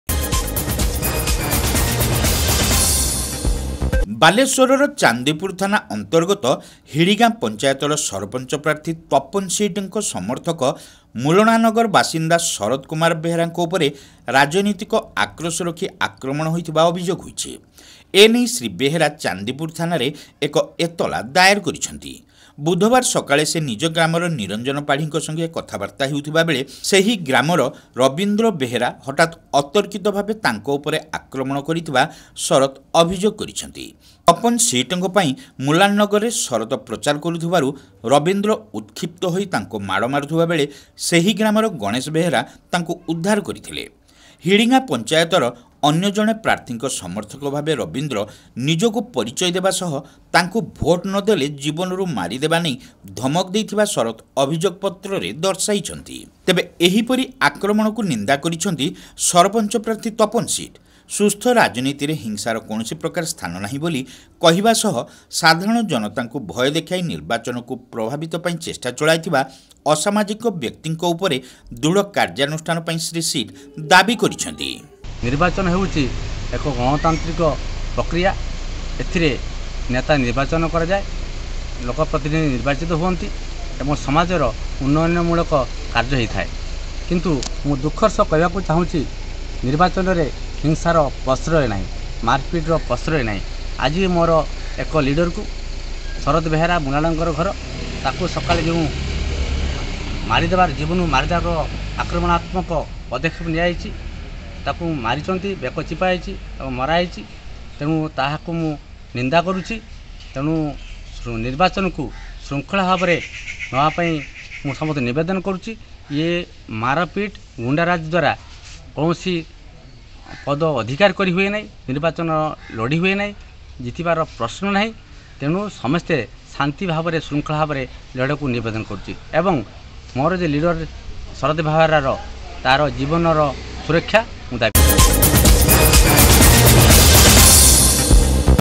ମାଧବ ନ୍ୟୁଜ - ବାଲେଶ୍ବରର ହିଡିଗାଁ ସରପଞ୍ଚ ପ୍ରାର୍ଥୀଙ୍କ ସମର୍ଥକଙ୍କୁ ମାଡ ଅଭିଯୋଗ
Madhab News - Balasore's Hidigan Sarpanch Candidate's Supporter Complains as He was Beaten - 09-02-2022